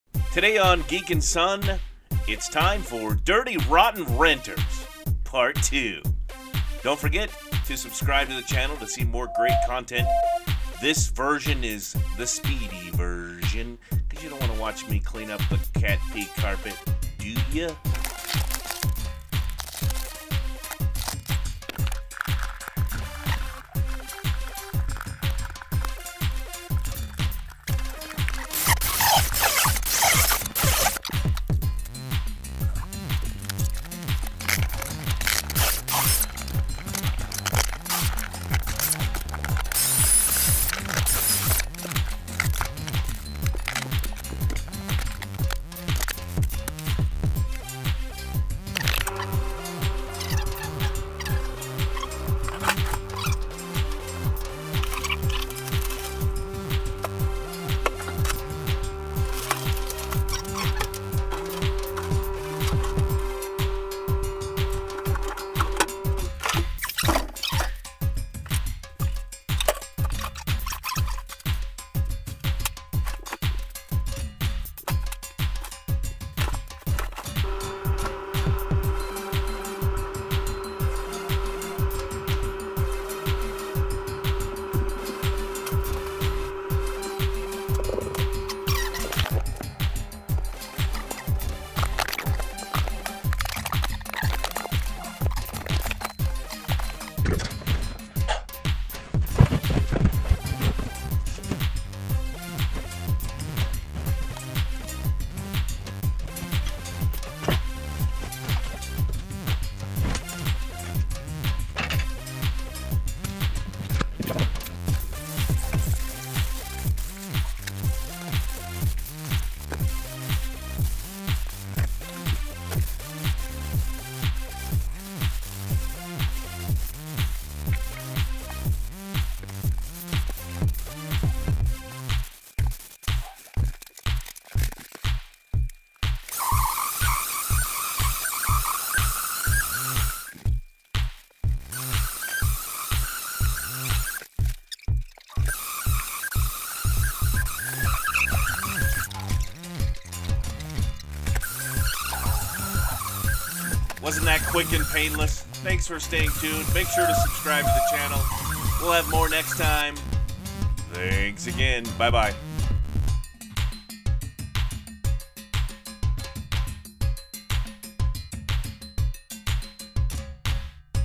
SPEEDY DIRTY ROTTEN RENTERS 2
Today on Geek and Son I repair damage done by the dirty rotten renters.
This is part two of the series where I repair all the damage.

Super Speedy and fun to watch!

Get a background check if you are renting a property!